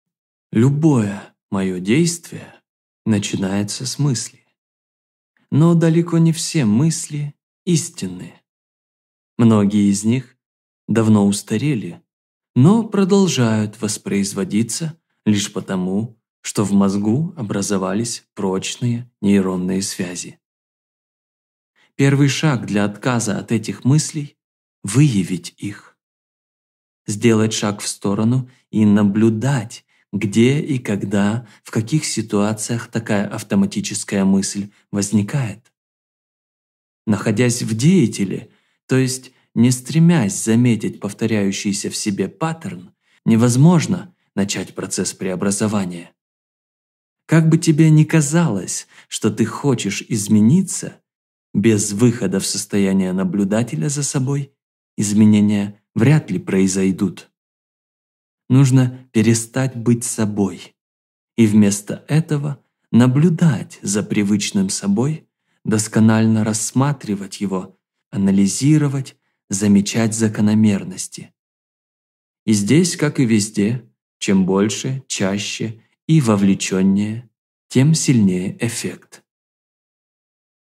Стань наблюдателем
Выйти из себя, перестать играть роль, отстраниться, взглянуть со стороны - это ключевое качество к изменениям, с которого, как мне кажется, всё и начинается.